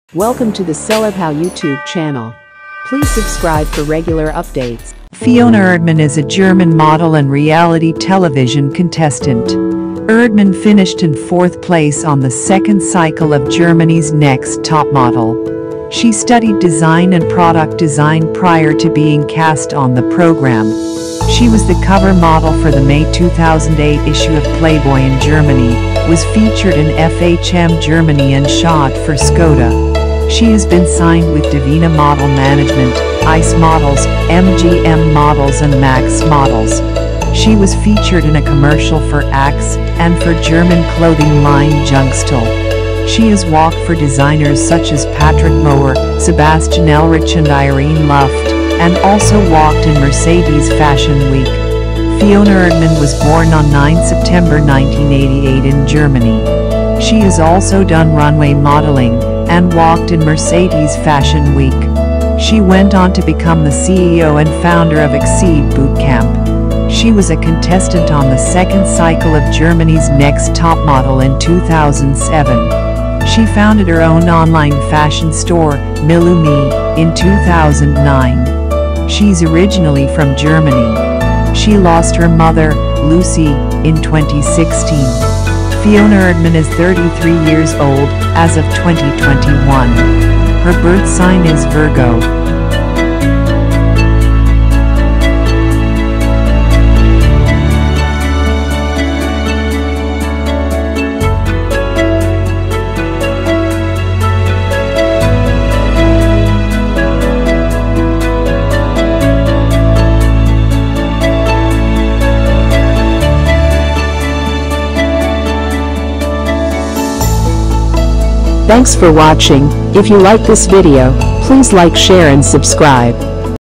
German Hot Model Fiona Erdmann Biography | Wiki | Age | Height | Net Worth | Lifestyle | Onlyfans
Fiona Erdmann is a German model and reality television contestant.  Erdmann finished in fourth place on the second cycle of Germany's Next

Topmodel. She studied design and product design prior to being cast on the programme. She was the cover model for the May 2008 issue of Playboy in

Germany, was featured in FHM Germany and shot for SKODA. She has been signed with Divina Model Management, Ice Models, MGM Models and Max

Models. She was featured in a commercial for Axe and for German clothing line Jungstil. She has walked for designers such as Patrick Mohr, Sebastian

 Ellrich and Irene Luft, and also walked in Mercedes Fashion Week.  Fiona Erdmann was born on   9 September 1988 in Germany.  She has also done

runway modeling and walked in Mercedes Fashion Week. She went on to become the CEO and founder of Exceed Bootcamp.  She was a contestant

 on the second cycle of Germany's Next Topmodel in 2007. She founded her own online fashion store, Milumee, in 2009.  She's originally from Germany.

 She lost her mother, Lucy, in 2016.  Fiona Erdmann is 33 years old as of 2021. Her birth sign is  Virgo.